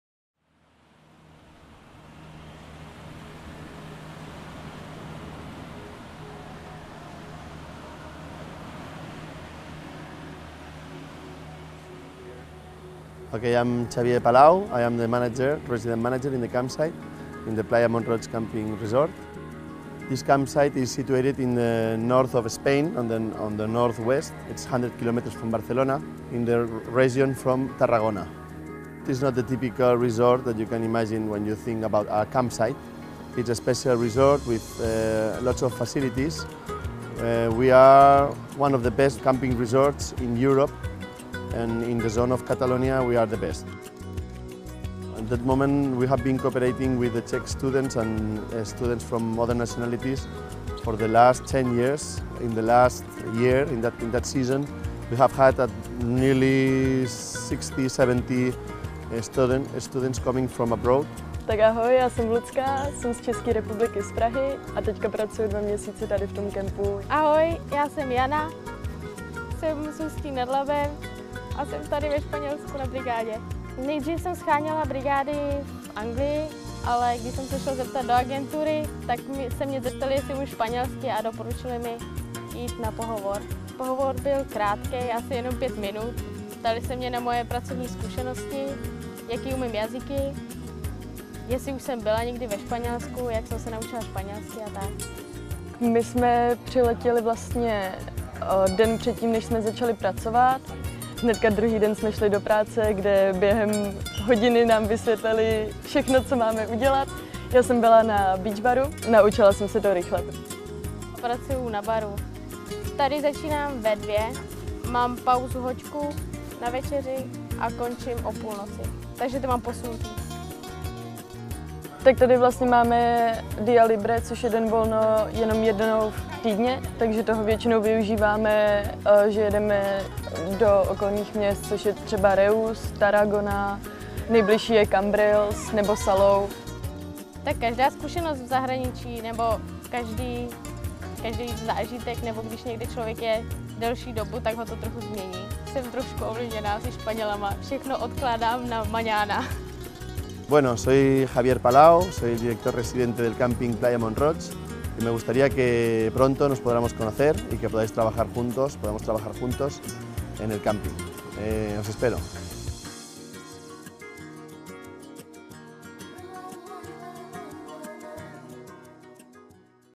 Brigáda ve španělském plážovém rezortu
Prázdninová brigáda ve Španělsku Vám umožní skloubit příjemné s užitečným - pracovat budete blízko pláže a po práci nebo ráno před směnou se můžete opalovat či si rekreačně zaplavat a přitom vyděláváte peníze.